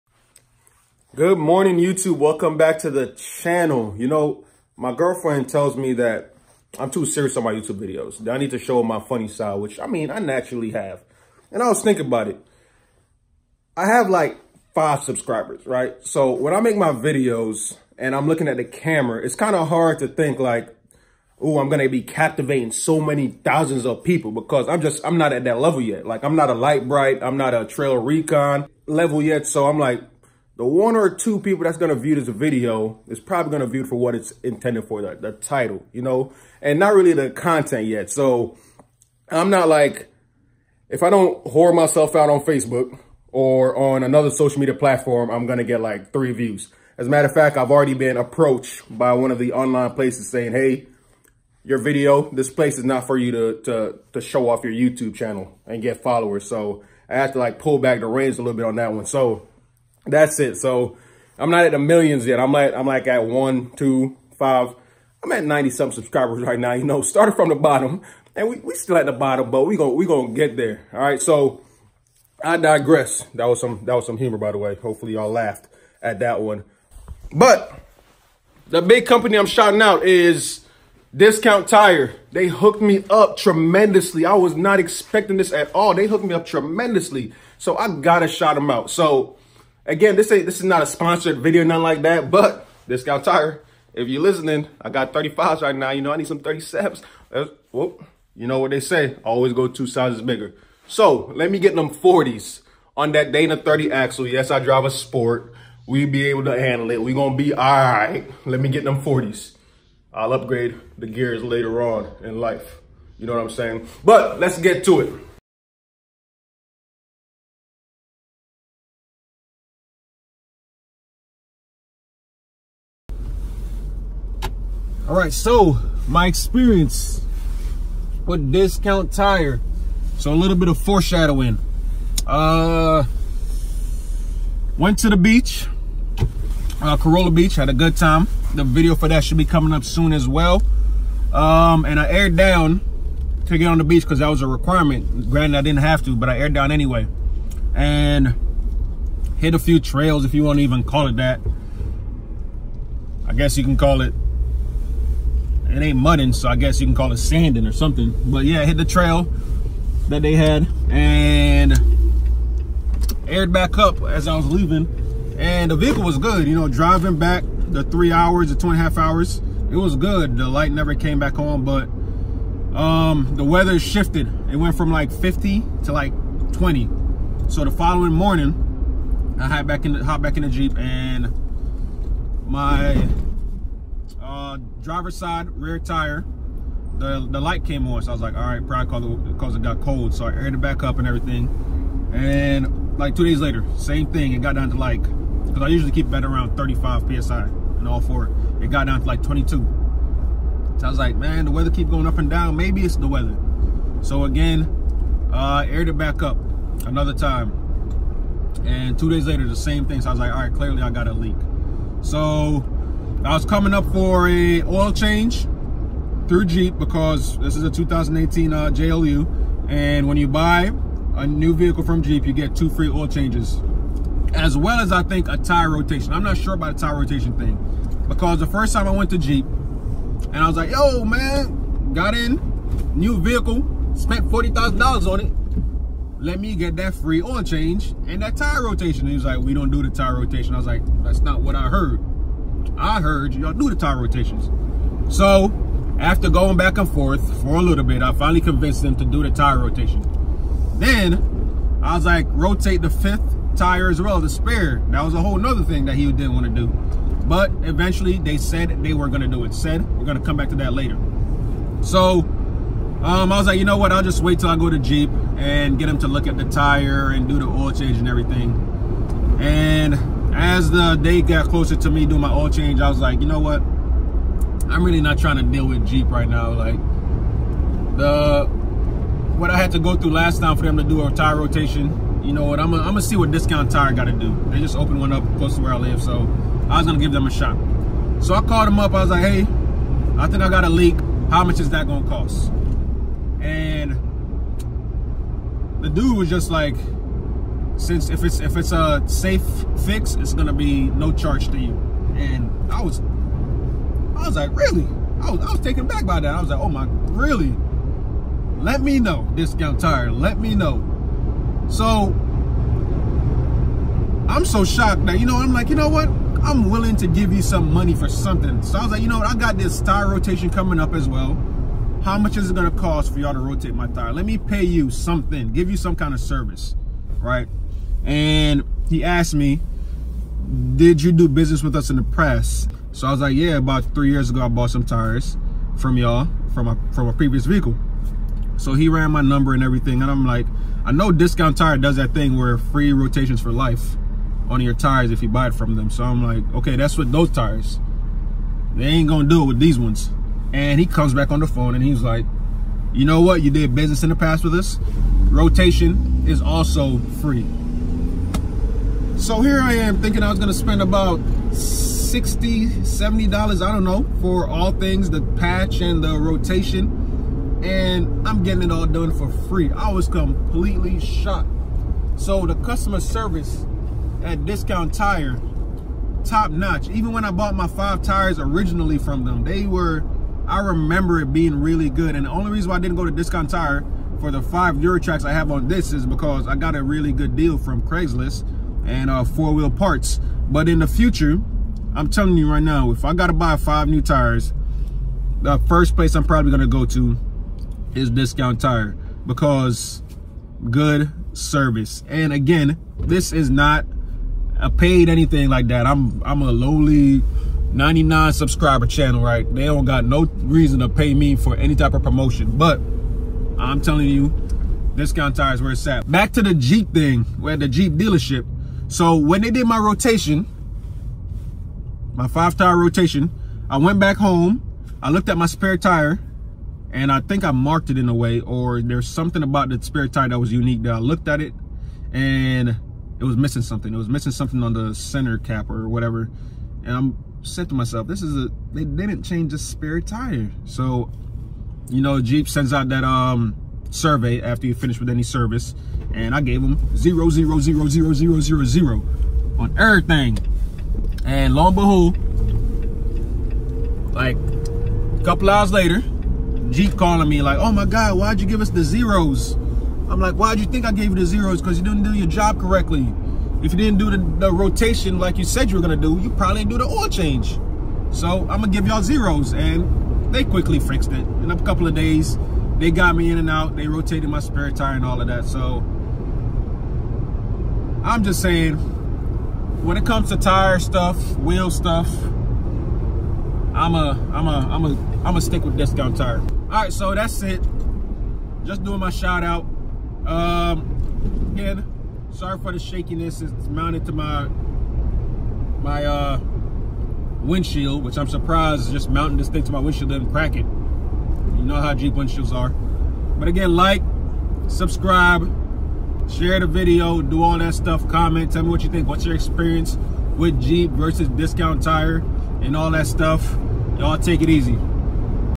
My Shocking Experience with Discount Tire
I was completely surprised about how discount tire treated me. The conversation I had one the phone with the rep was the last thing I expected.